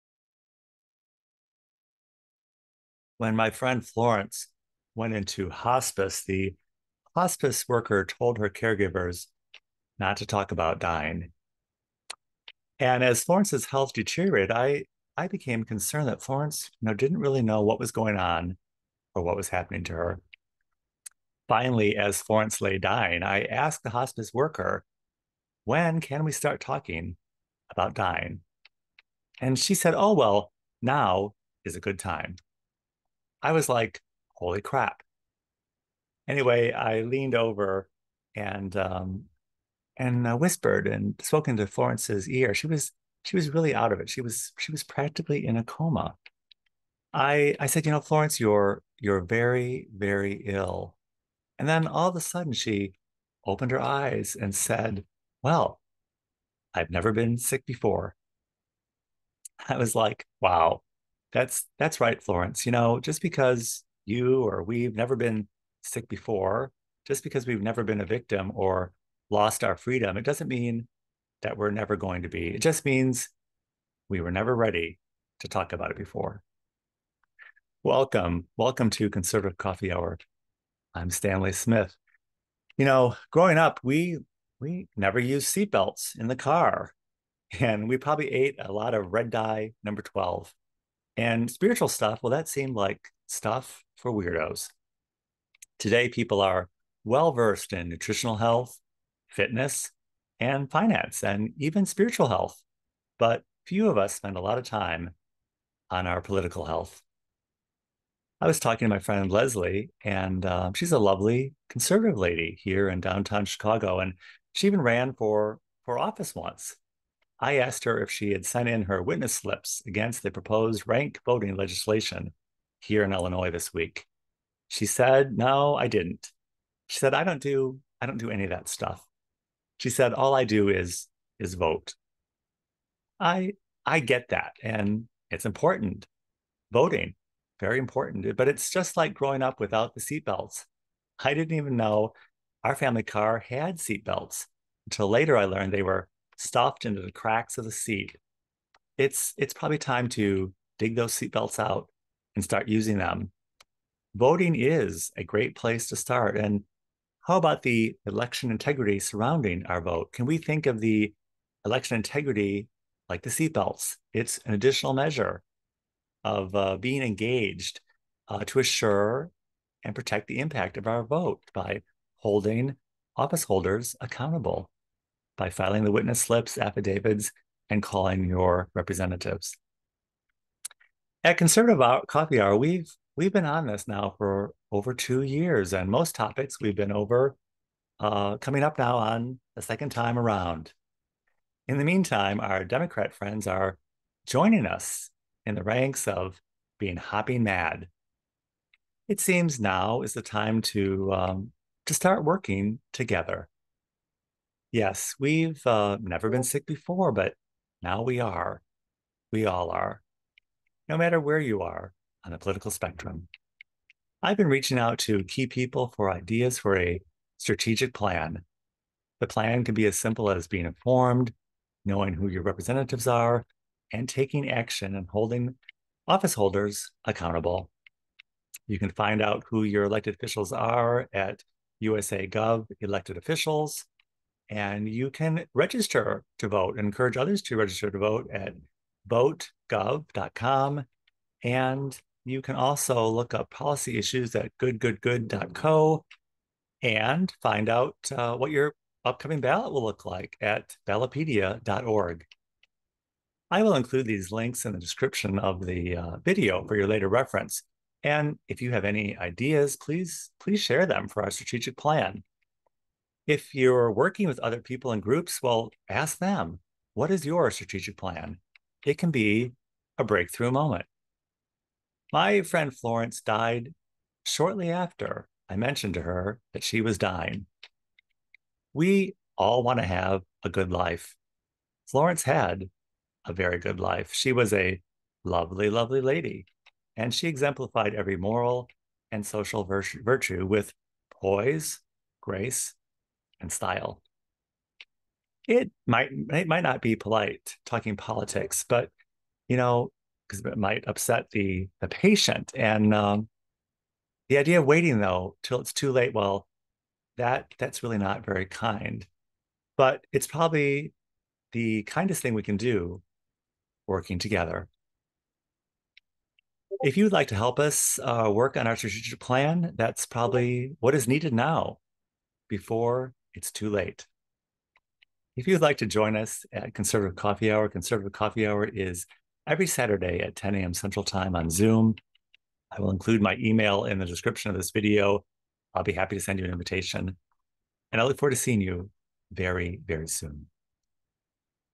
Working Together
We never needed to talk about it or do anything before to assure our freedom but now we do. Working together we can vote and have election integrity and hold people who hold office accountable.  This week we look at starting a strategic plan to move from complaining and fighting with each other to working together. To join us at Conservative Coffee Hour please email . Conservative Coffee Hour is every Saturday at 10:00 am Central Time on Zoom.
STRATEGIC PLAN INITIATIVE
Let's develop a strategic plan, individually and together to define where we've been, where we're going and how to get there.